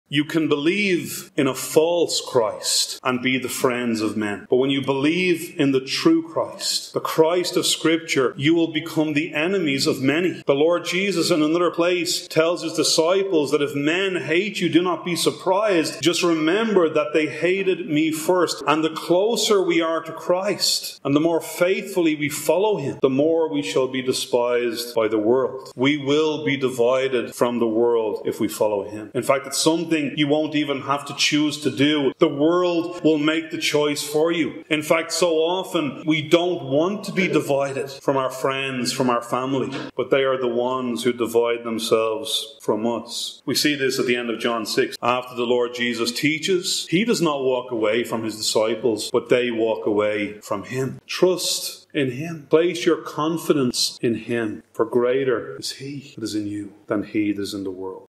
You Will Become the Enemies of Many - Mark Fitzpatrick Sermon
Matthew 24:9
9 “Then they will deliver you up to tribulation and kill you, and you will be hated by all nations for My name’s sake."

John 6:66 From that time many of His disciples went back and walked with Him no more.

▶️You can help support Arann Reformed Baptist Church, a most blessed, Christ-centered church ministry, at the following